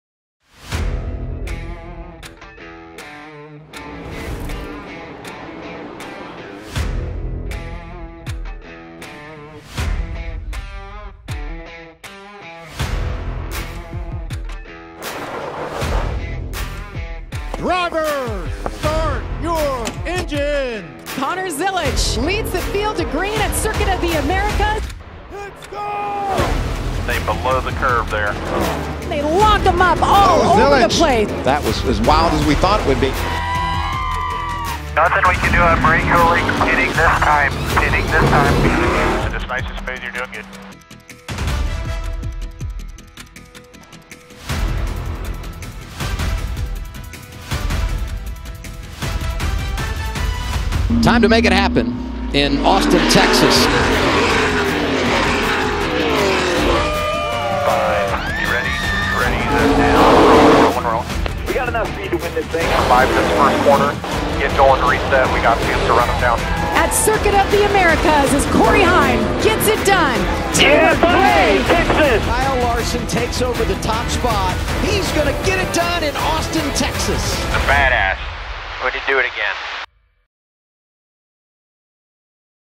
Doubleheader Day at the Circuit
A full day of racing at Circuit of The Americas. Fans, Family, Fun. Doesn't get much better than that.